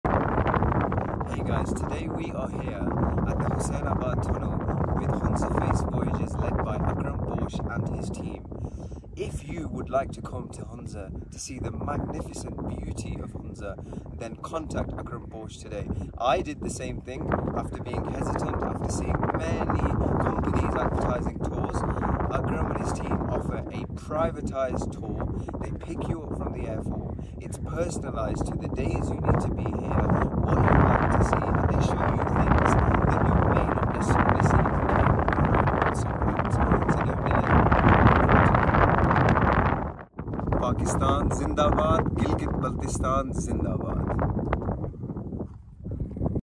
Customize your toursPlan a Voyage, Make it Memorable WhatsApp +923445899628Instagram
Mr. Kami and his Friend who visited Pakistan after 14years from UK books a customized tour with us @hunzaface. We are delighted to serve our clients. They booked their tour for Skardu,Gojal Hunza, and Islamabad. They were so amazed by witnessing the enchanting beauty of the North.They were happy to glare at the mesmerizing beauty of Hunza & Skardu They enjoyed the quality time with us.
They were satisfied with our assistance.
Your satisfaction is a great compliment,
PAKISTAN ZINDABAD 🇵🇰
Customize your tours
Plan a Voyage, Make it Memorable